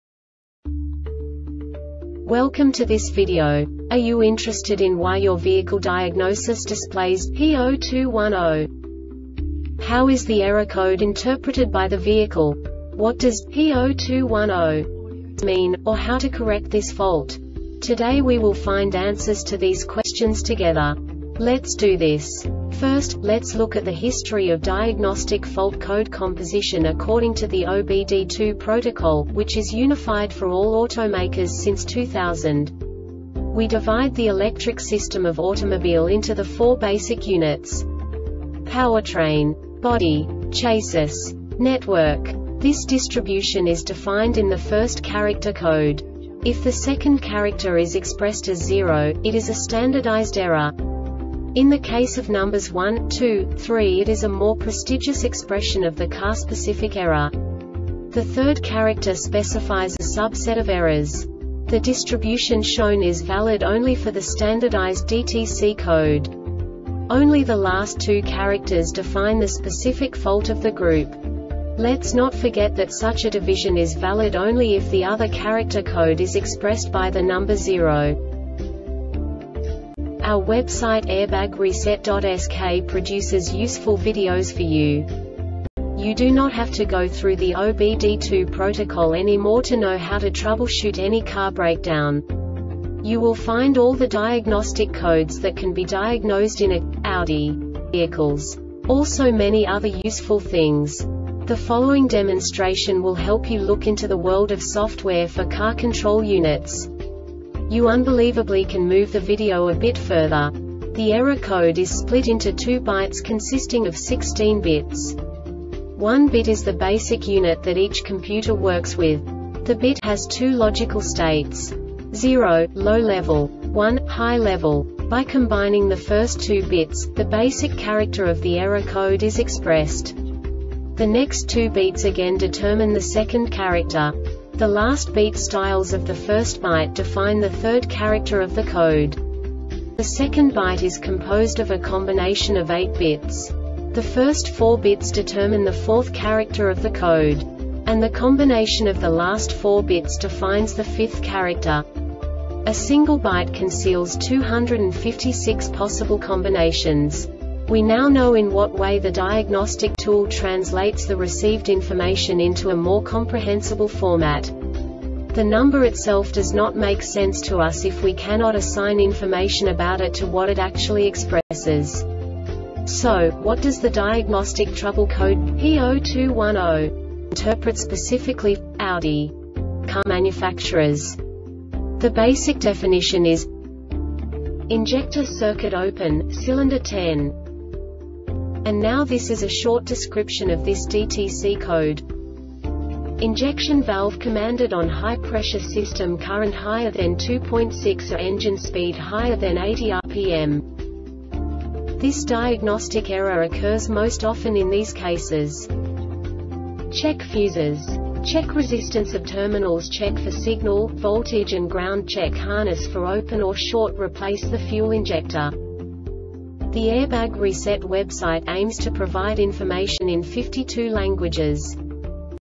DTC Audi P0210 Short Explanation
Contents:
0:21 Basic DTC analysis according to OBD2 protocol standard.
1:48 Insight into programming
2:58 A diagnostically understandable description of the Diagnostic Trouble Code
3:05 Basic error definition

Make:
Audi
Code:
P0210
Definition:
Injector Circuit / Open - Cylinder 10
Description:
injection valve commanded on high pressure system current  higher then 2.6 A engine speed  higher then 80 RPM
Cause:
Check fuses.
Check resistance of terminals
Replace the fuel injector.